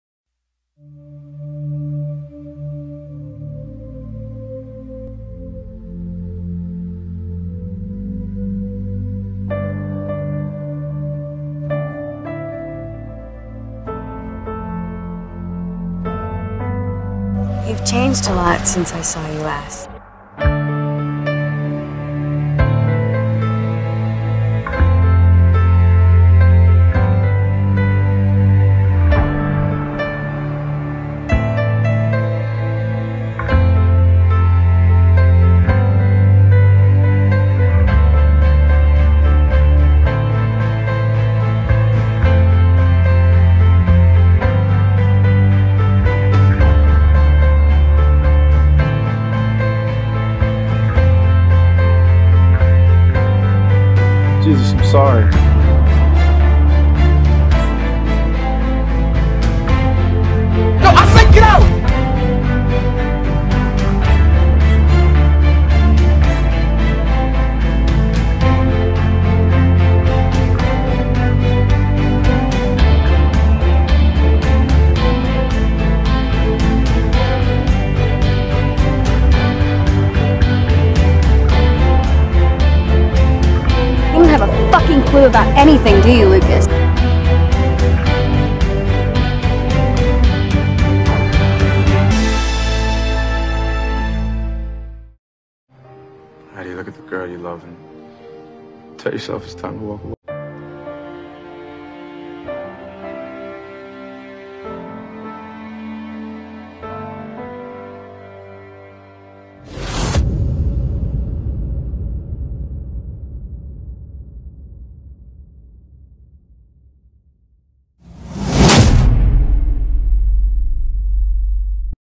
Vallen Anne Provoost
Dit is een nagemaakte trailer van het boek "Vallen" van Anne-Provoost, met fragmenten van Rachel McAdams en Channing Tatum als Lucas en Caitlin.